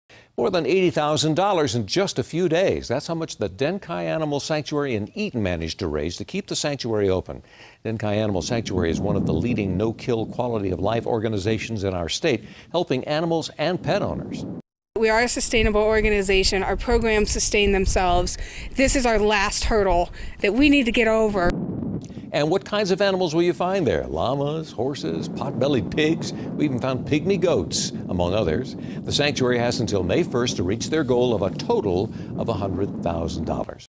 Denkai Animal Sanctuary aims to raise $200,000
Denkai Animal Sanctuary, one of the leading no-kill organizations in the state, is trying to raise $100,000.